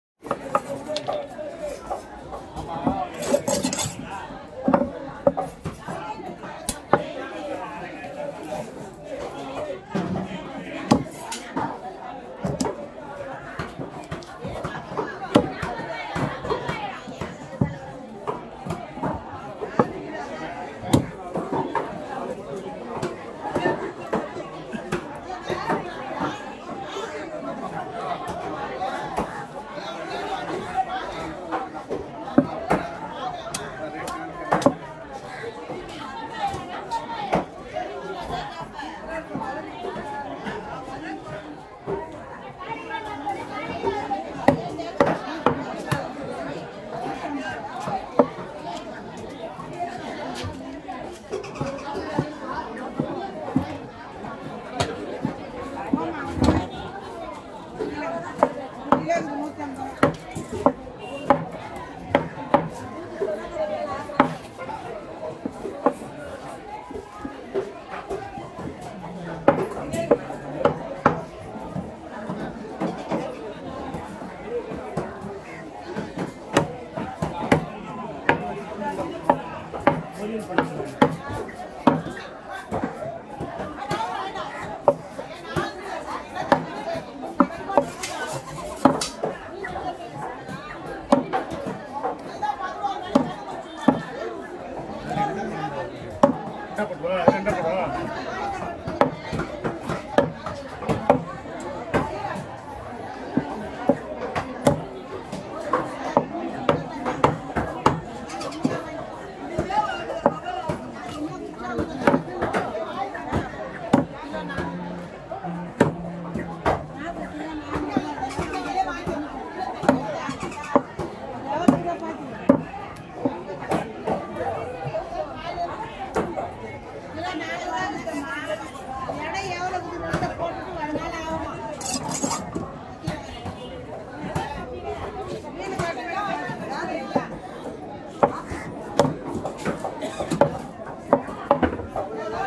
TREVALLY FISH CUTTING 🔪🔥
THIS VIDEO WAS TAKEN AT KASIMEDU FISH MARKET